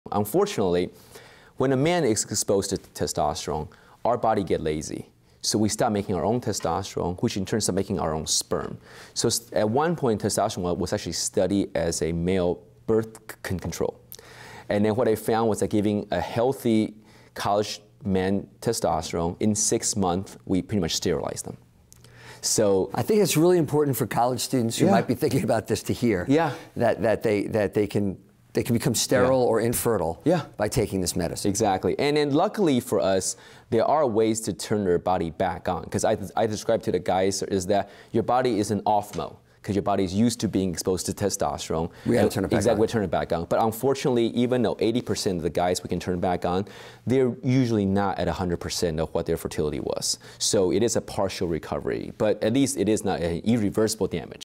Testosterone & Infertility Danger
Dr. T. Mike Hsieh joins host Dr. David Granet to discuss what you should know about the dangers of too much testosterone in healthy males and its impact on fertility.